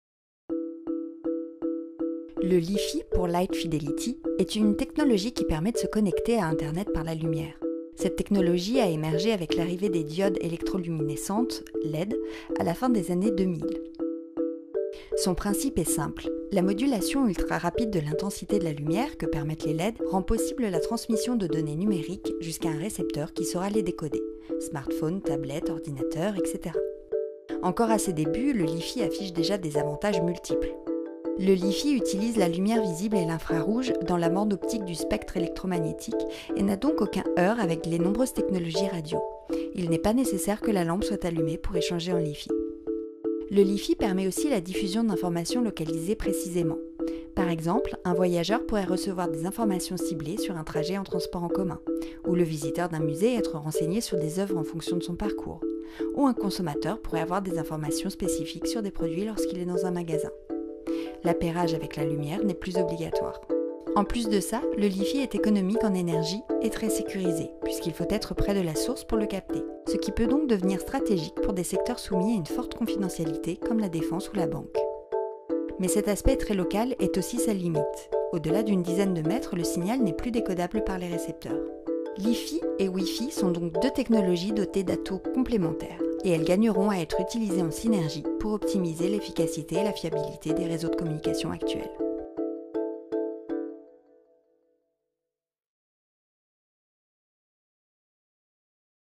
La technologie Li-Fi - Explications - Comment ça marche - Video Orange
Explication du fonctionnement et des applications possibles de la technologie Li-Fi.